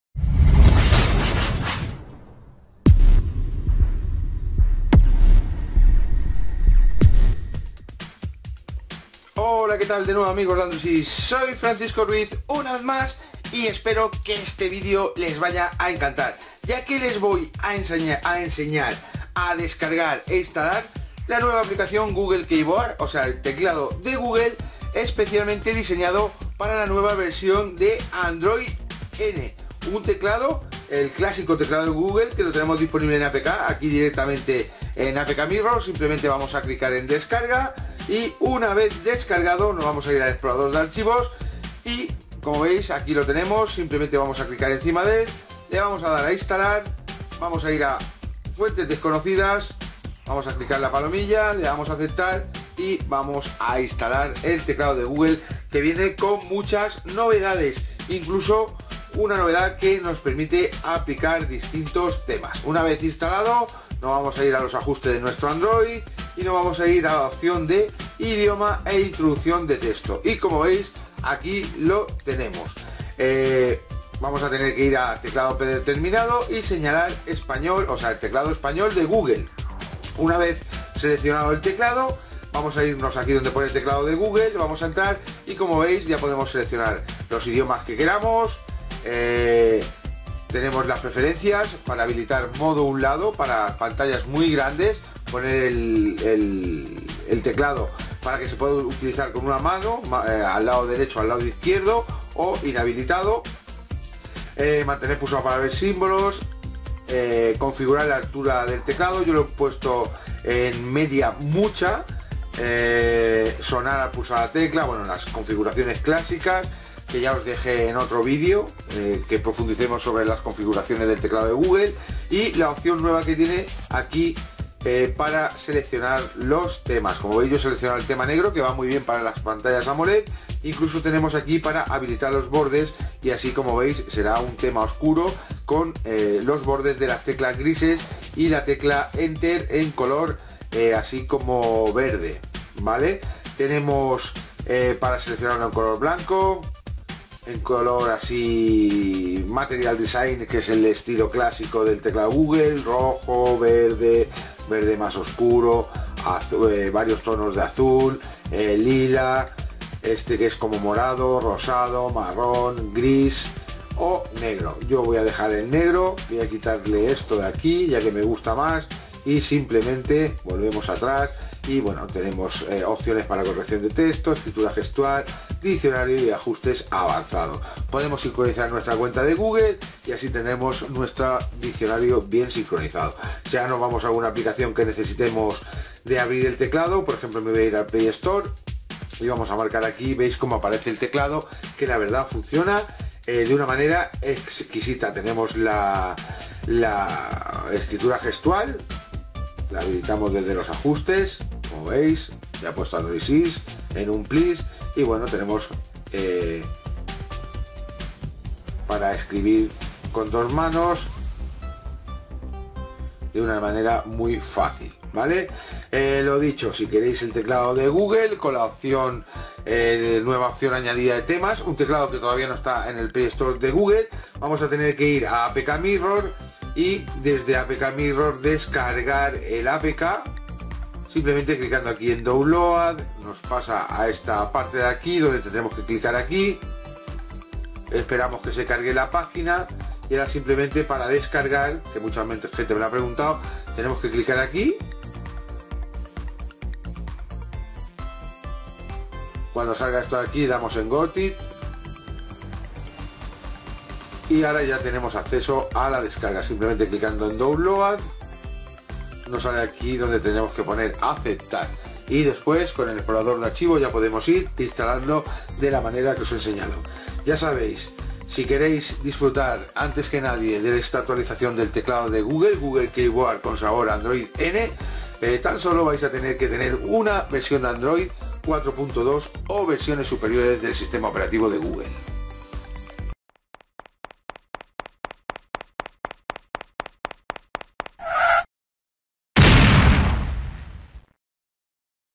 [APK] Descarga ya el nuevo Teclado de Google de Android N
¿Eres un adicto a probar las nuevas aplicaciones antes de que incluso se lancen de manera oficial al Play Store de Google?. Si la respuesta a esta pregunta es un SI rotundo, sin duda alguna estás de enhorabuena ya que te vamos a facilitar la descarga en apk del nuevo teclado de Google de Android N, un teclado de Google ya clásico y éxito en descargas el cual te traemos en su última actualización oficial mucho antes de que esta esté en la tienda oficial de aplicaciones para Android el Google Play o Play Store de Google.

En el vídeo adjunto de la cabecera de este artículo, a parte de enseñarte una de las principales peculiaridades o novedades de este nuevo teclado de Google de Android N, una nueva funcionalidad que nos permite aplicar varios y atractivos temas de diferentes colores, también os explicamos el sencillo proceso para la descarga del APK así como el sencillo método de nueva instalación del Teclado de Google o Google Keyboard o la actualización del mismo.